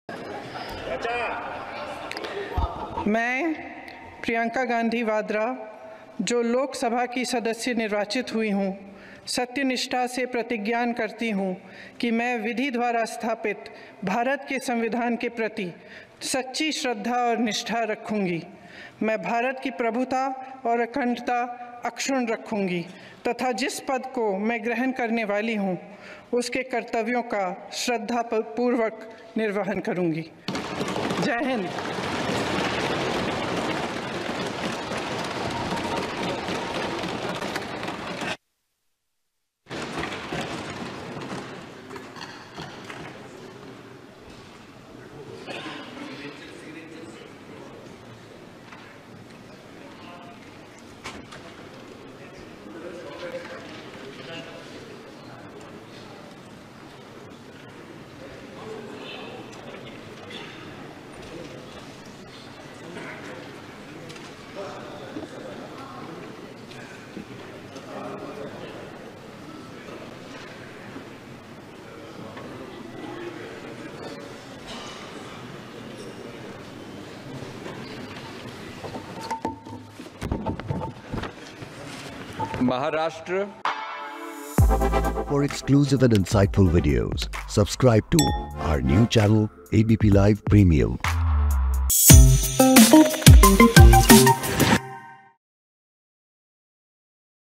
Priyanka Gandhi Oath Ceremony: मैं प्रियंका गांधी वाड्रा... प्रियंका गांधी ने ली सांसद पद की शपथ
शपथ लेने के बाद एबीपी न्यूज़ का प्रियंका गांधी से सवाल की क्या प्राथमिकताएं रहेगी तो प्रियंका ने कहा अपने देश के लिए अपनी पार्टी के मुद्दे उठाऊंगी